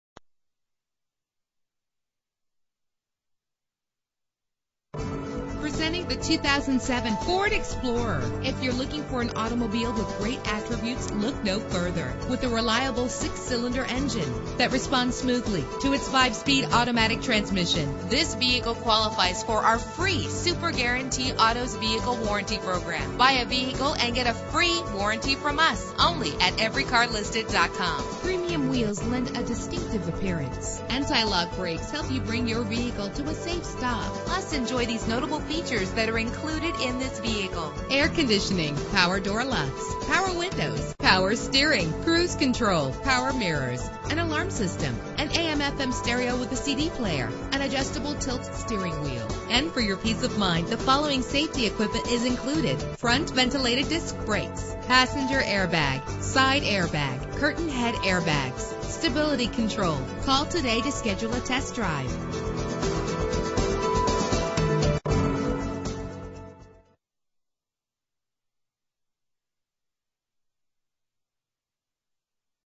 2007 Ford Explorer - Fort Worth TX
Year: 2007
Make: Ford
Model: Explorer
Trim: Eddie Bauer
Engine: 4.0 liter 6 cylinder 12 valve
Transmission: 5-Speed Automatic
Color: Maroon
Mileage: 45152
Address:
9101 Camp Bowie West